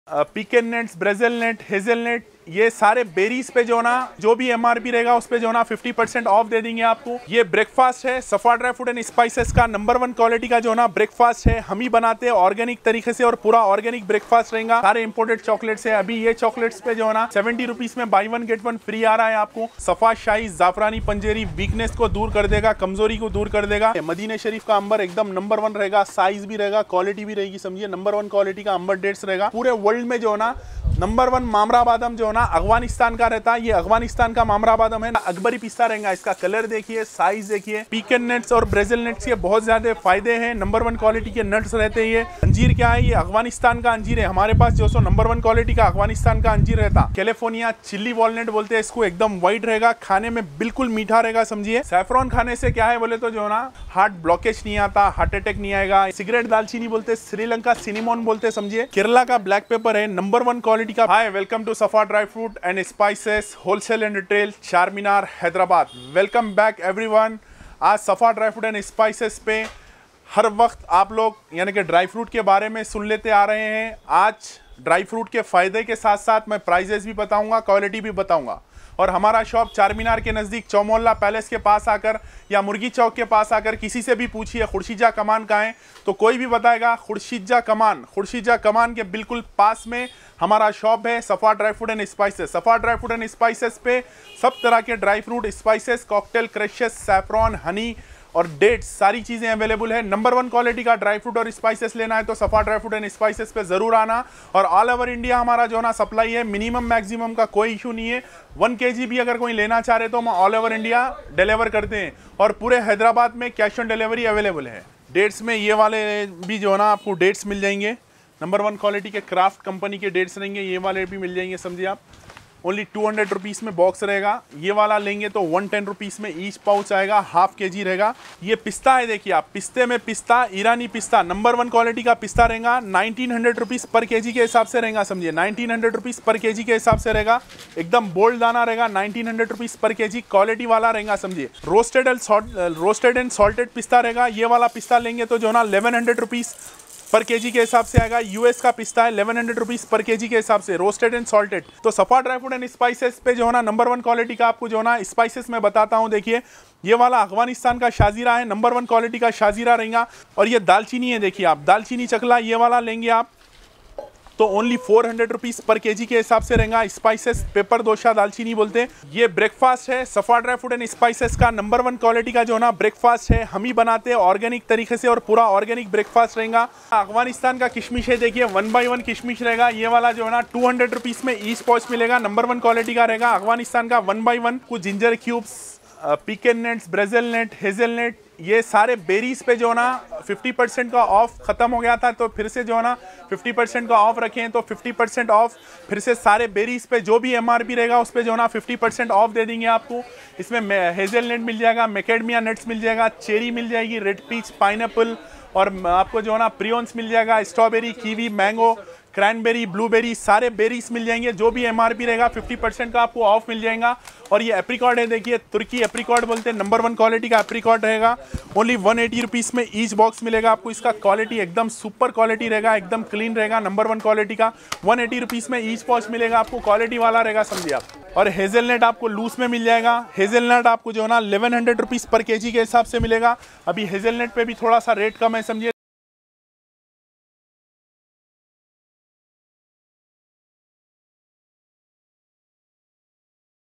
Kashmir, Afghani Wholesale Dry Fruits In Hyderabad | Safa Dry Fruits and spices charminar hyderabad
Safa Dry Fruits & Spices
20-3-336, Near Tariqua Manzil
Khursheedjah, Devdi, Shagunj,
Charminar, Hyderabad - 02